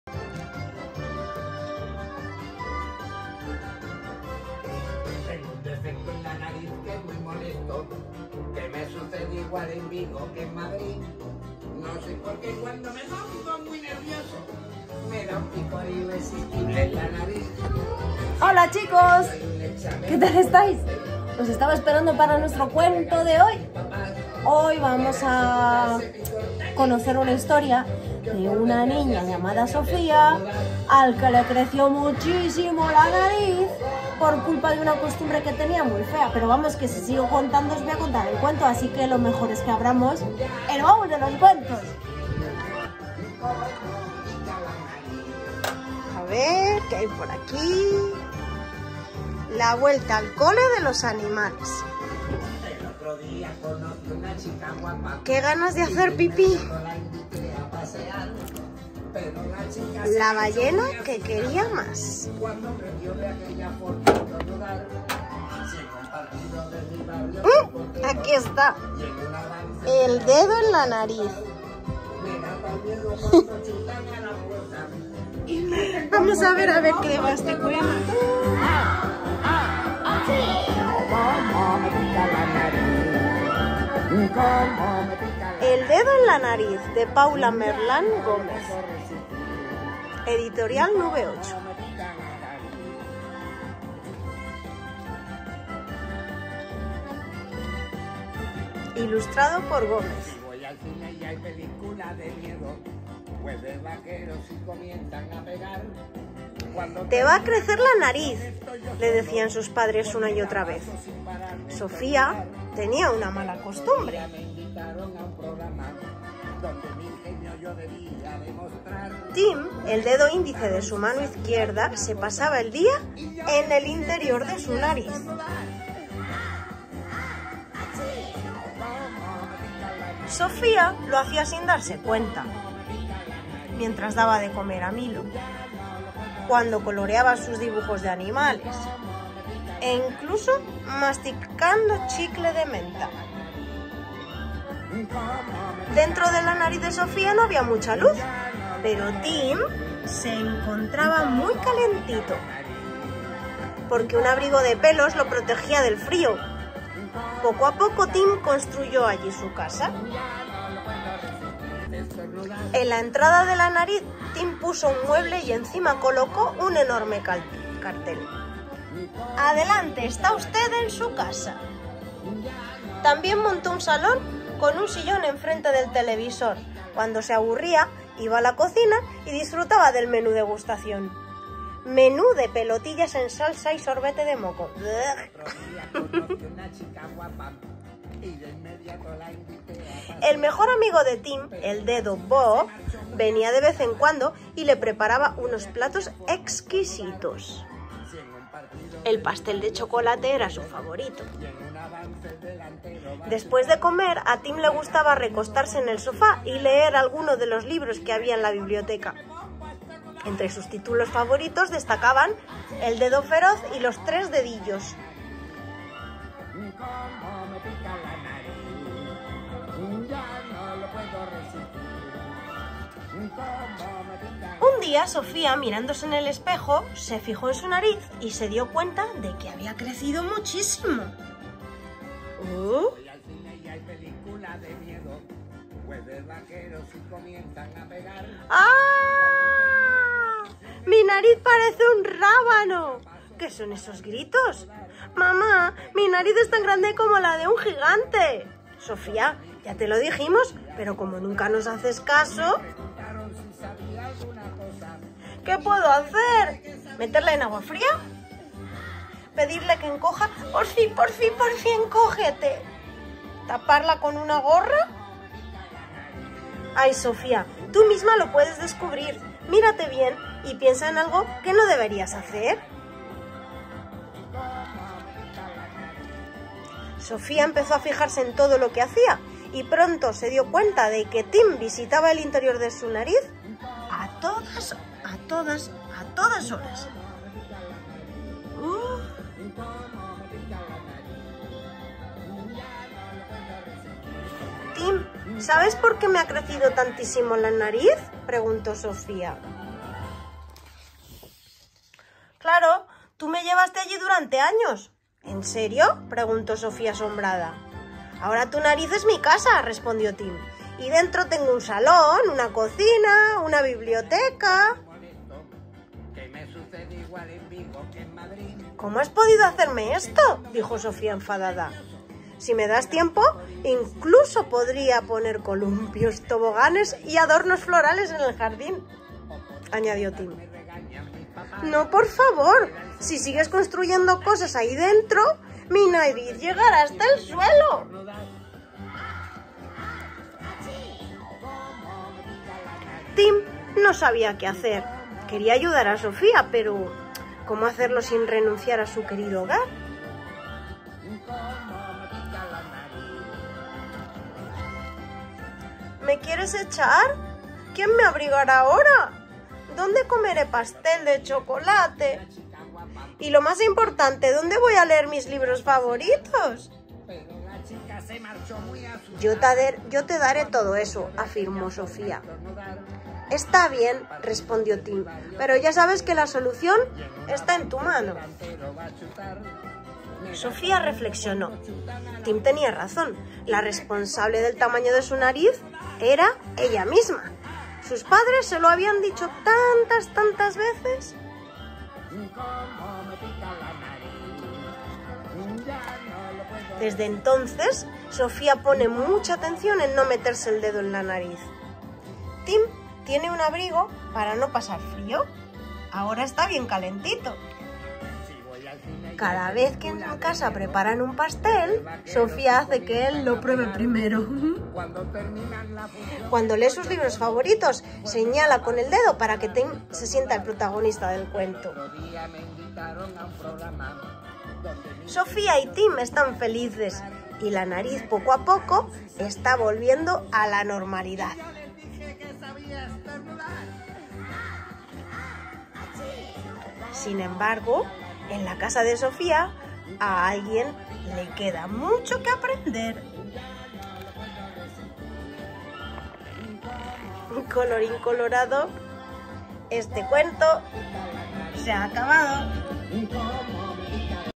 El dedo en la nariz
Autora: Paula Merlán
Ilustrador: Gómez
Editorial: Nuebocho

Meterse el dedo en la nariz puede tener efectos secundarios. Una divertidísima historia de Paula Merlán y Gómez.

¡A Sofía le ha crecido le nariz! Ya se lo habían dicho sus padres. Y es que, esta niña no para de meterse el dedo en la nariz. Tanto, que su dedo se ha hecho una verdadera casa allí dentro: un salón, una cocina con menú degustación…